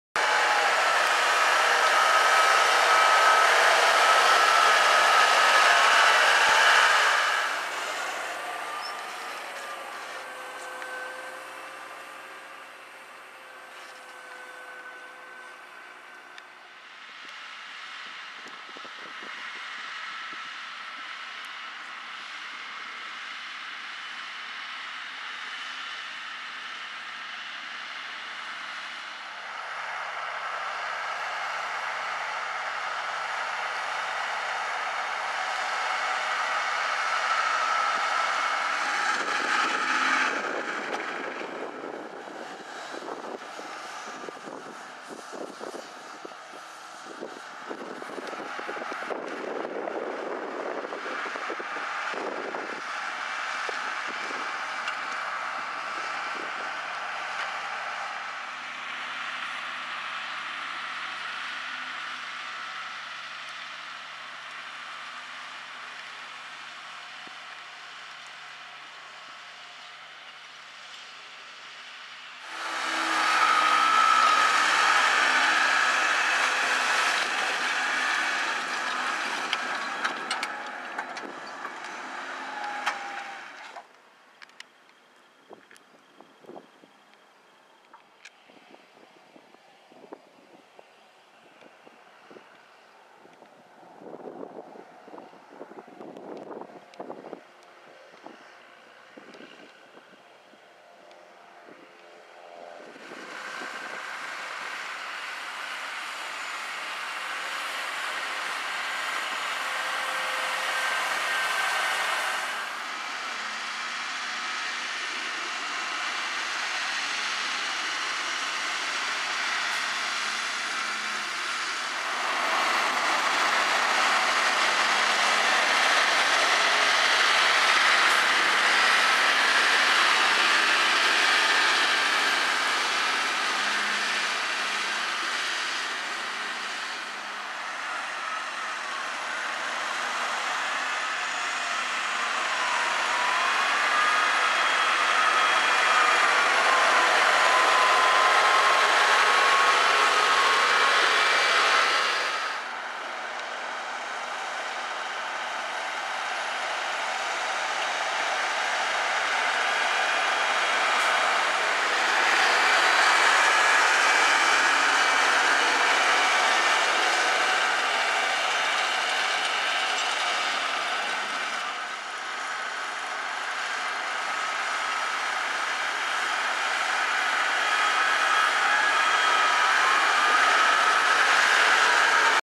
Slurry Injection | New Holland | Schuitemaker | Nunspeet | van Klompenburg
Loonbedrijf van Klompenburg uit Hulshorst in Nunspeet.
Het injecteren gebeurd met een New Holland Blue Power T7.270 met een Schuitemaker Robusta Perfekta 2 asser.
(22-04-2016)